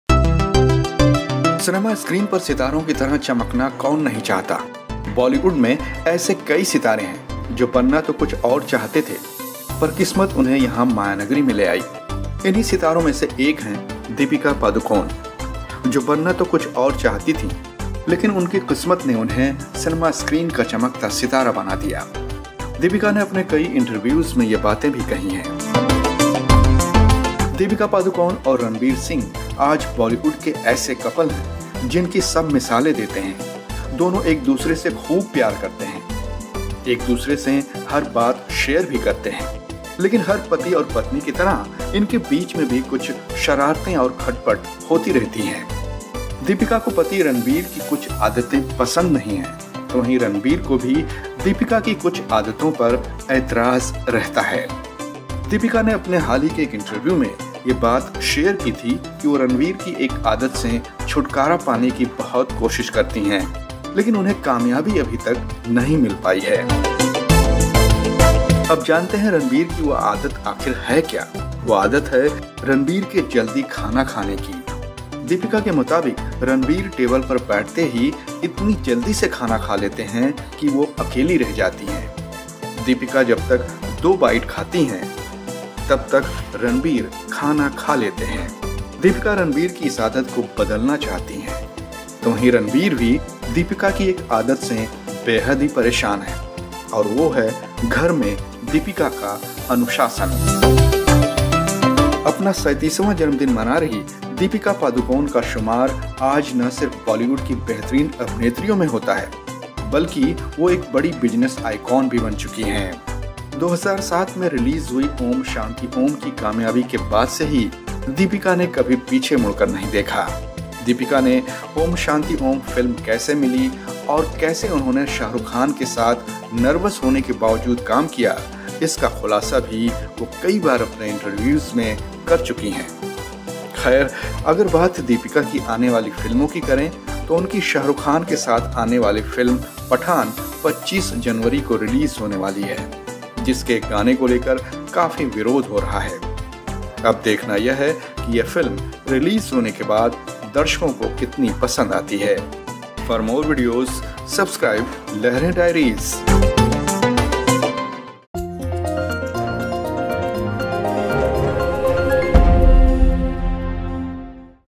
Deepika Padukone And Ranveer Singh Both Wanted To Change This BAD Habit | Lehren Diaries
Bollywood Actress Deepika Padukone आज अपना 37वां Birthday Celebrate कर रही हैं | इस मौके पर वो अपने पति Ranveer Singh की इस आदत को बदलना चाहती हैं जबकि Ranveer Singh भी कुछ ऐसा ही चाहते हैं.To know more,watch this video.